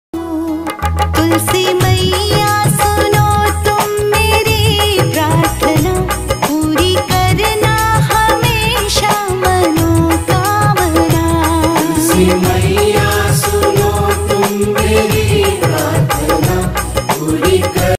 तुलसी पूजन🙏🙏 Tulika, Stales
hello everyone welcome back to tulika 'stales kase ho app sab ihope app acche honge is video me Mane Tulsi pujan dikhaya hai